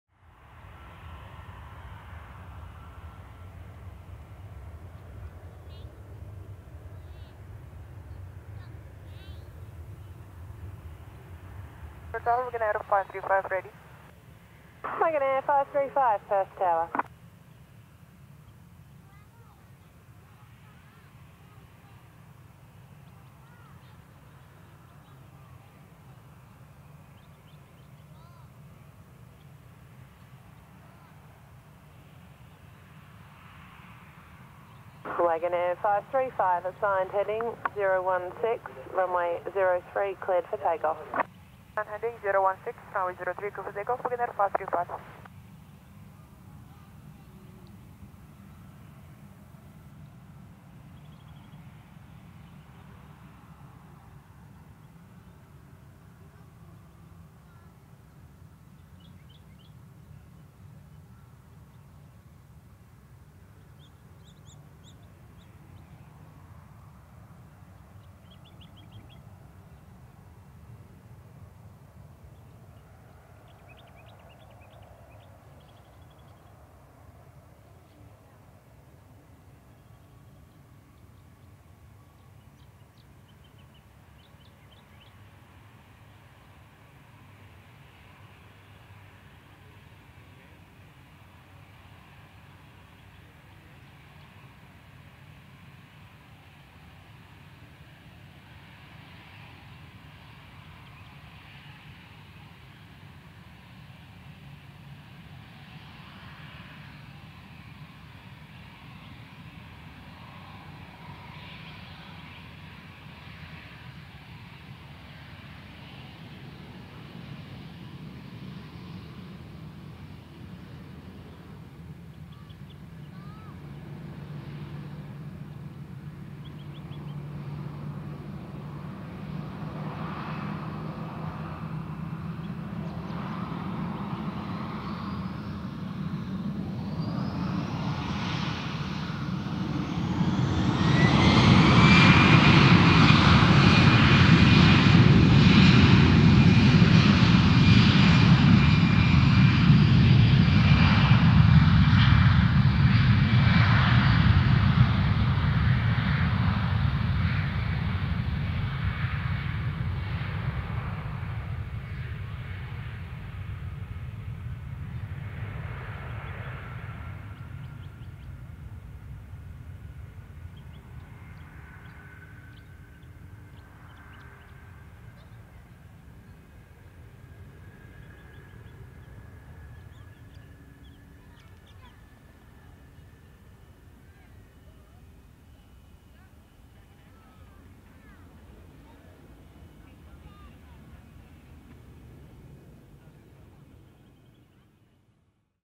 AirAsia Indonesia (PK-AXX) departing RW03 at Perth Airport.
AirAsia Indonesia (PK-AXX) Airbus A320-216 departing RW03 at Perth Airport.